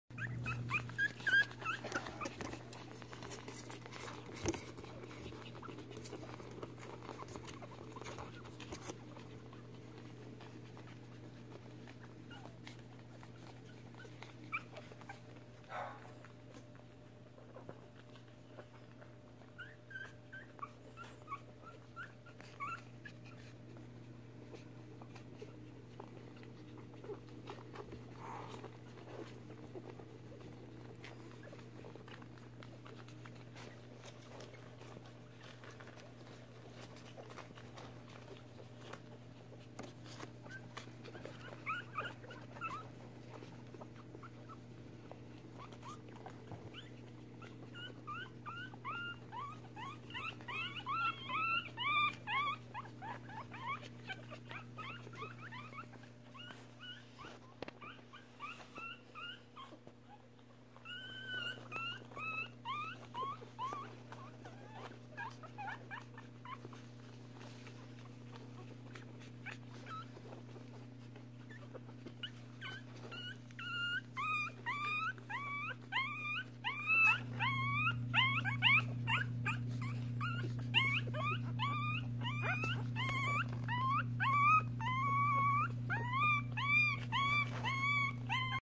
Mr. Navy, super chill guy. 🤗
Vixen x Victor babies. Almost 2 weeks old.